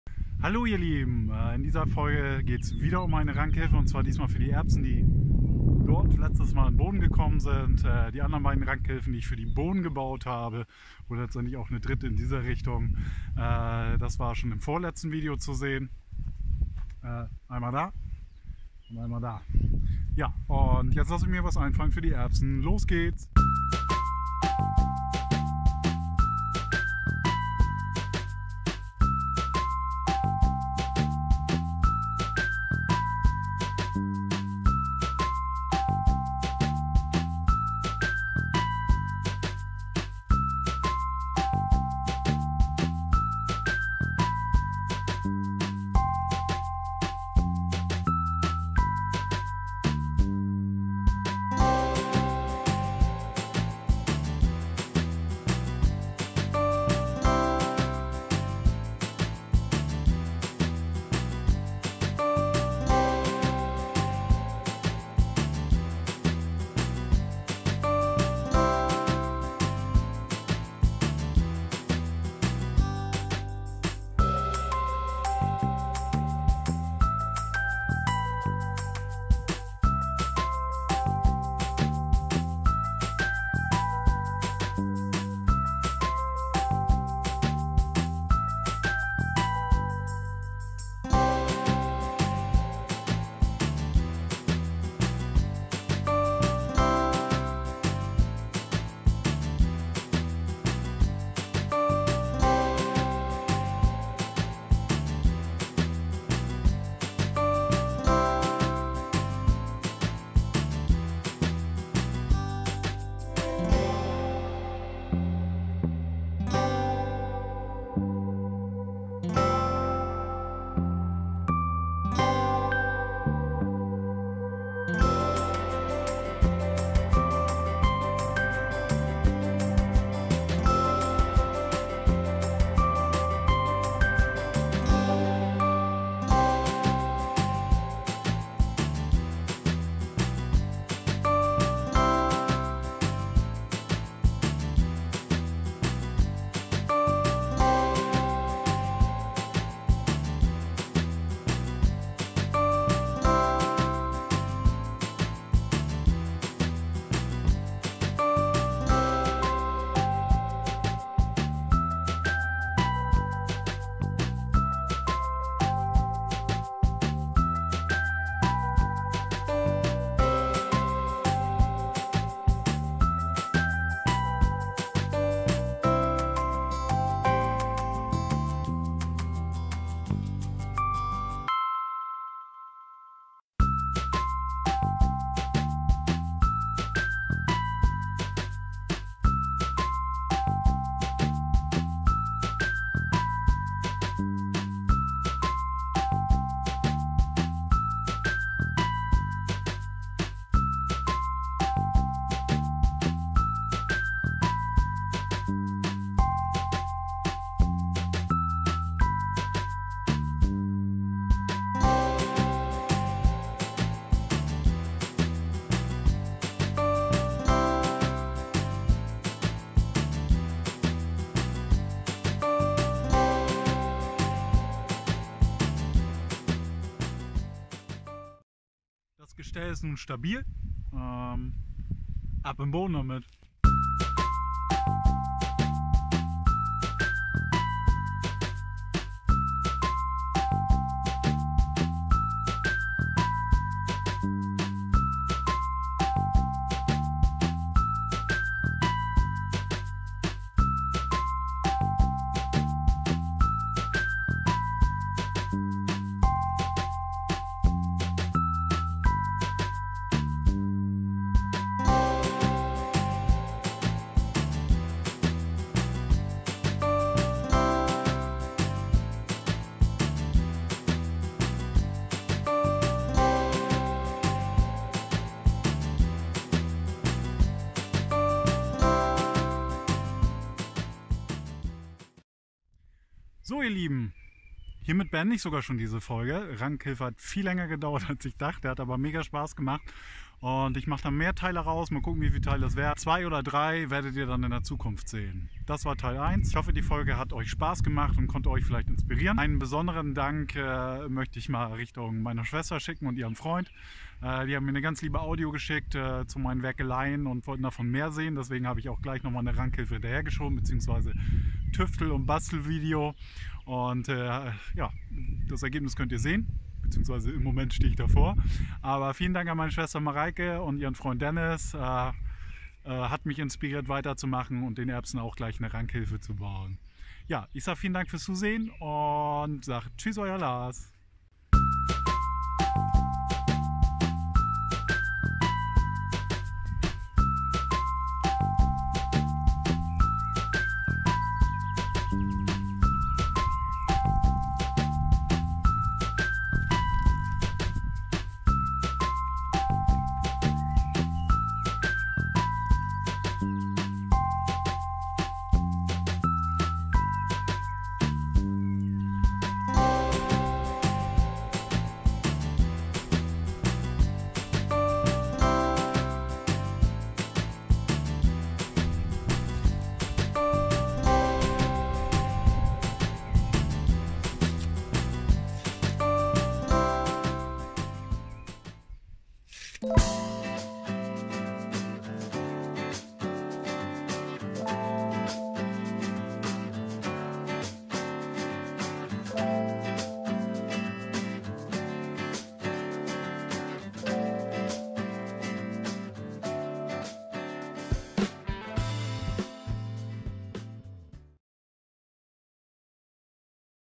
Rankhilfe selber bauen Variante 2 DIY Erbsen Folge 41
Erbsen Rankhilfe selber bauen? Ich mach mich daran und tüftel an den Rankhilfen für meine Erbsen. 3 verschiedene Rankhilfen werden es und die Konstruktion der 1. seht Ihr in diesem Video. Bis zum Ende schauen lohnt sich!
Euer Lars_()_